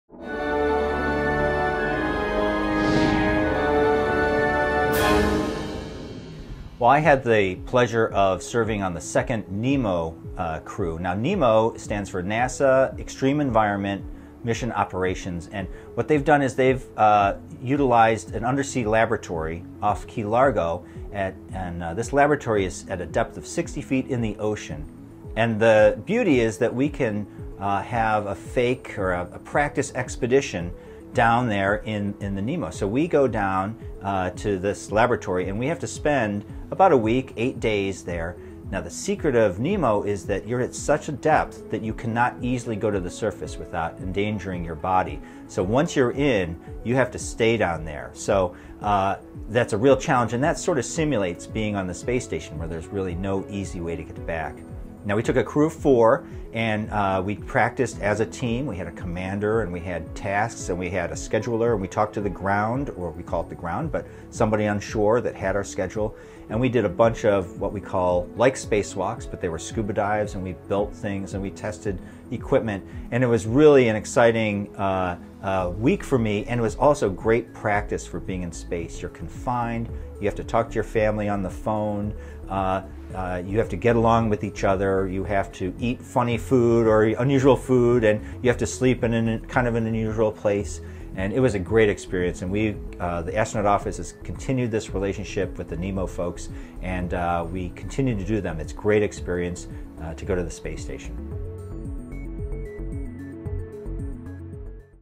Tell Me A Story: What is a NEEMO Mission?
Former NASA astronaut Dan Tani shows us why astronauts do extended training in a deep sea rig called NEEMO, on this Tell Me a Story.